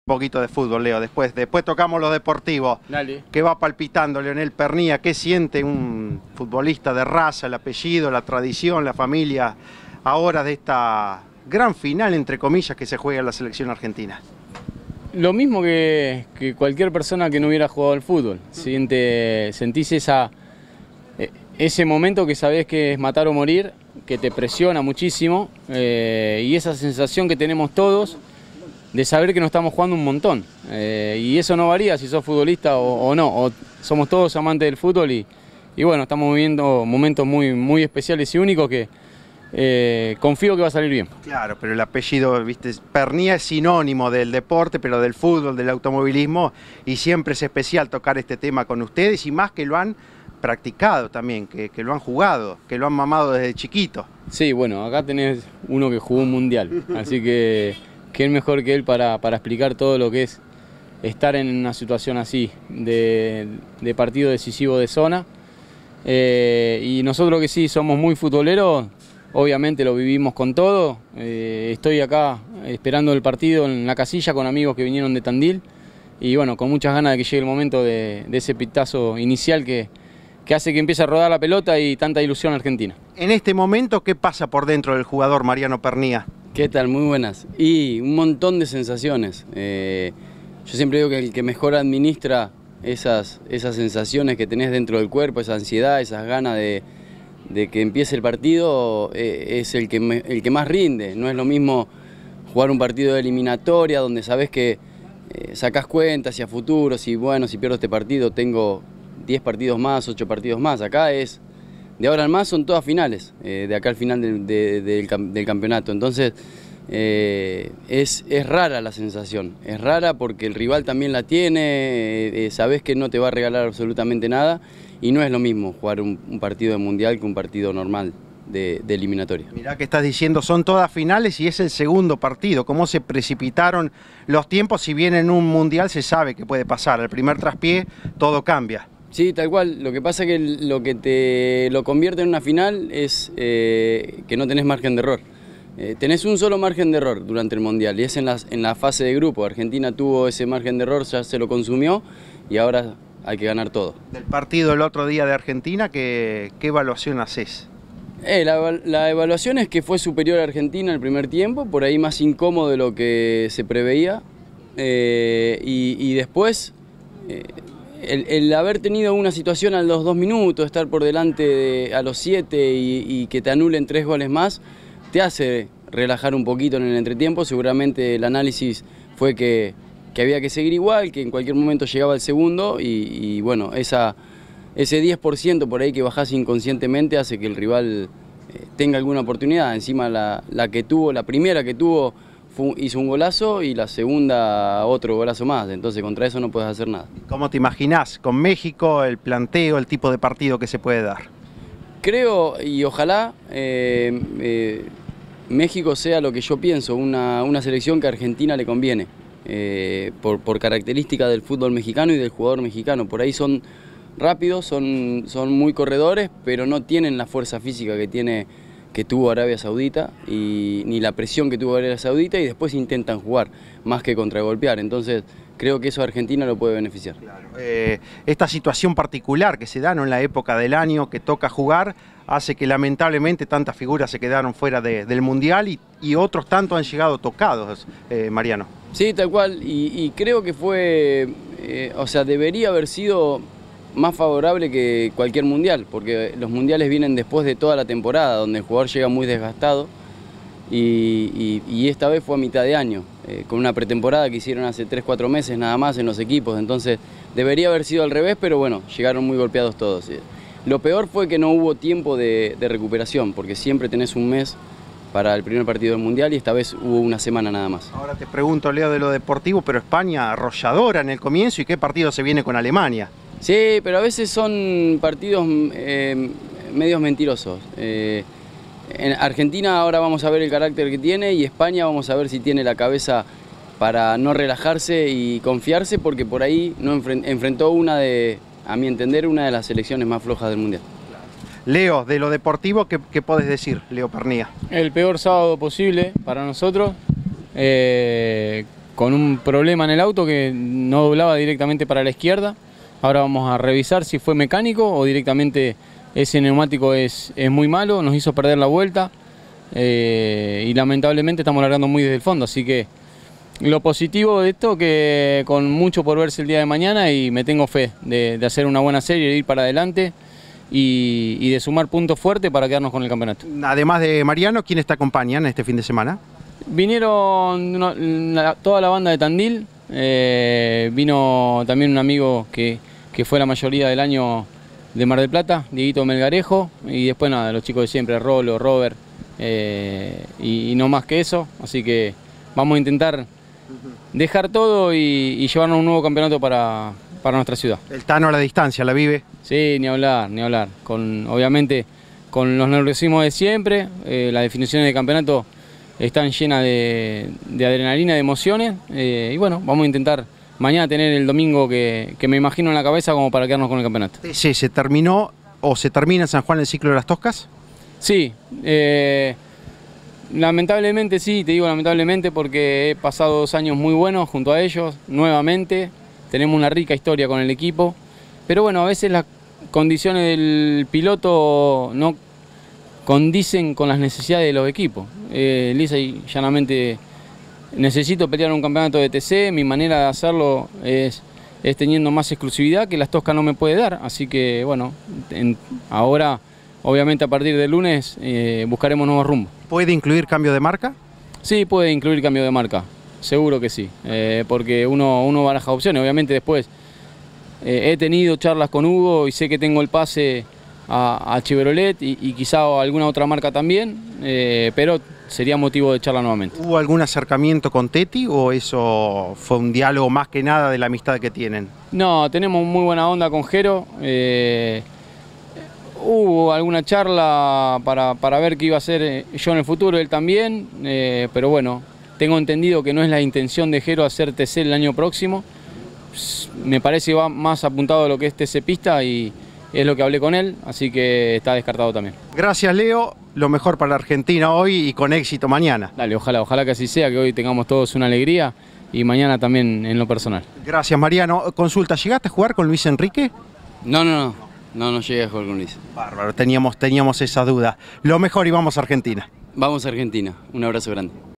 Leo Pernía: Cambia en TC, pelea en TN y el Mundial de fútbol
Los hermanos Leonel y Mariano Pernía hablaron de todo antes de cerrar el año con el TN en Rosario.  Leo habló del nuevo camino en TN y los dos  hablaron con el corazón sobre el Mundial de Qatar.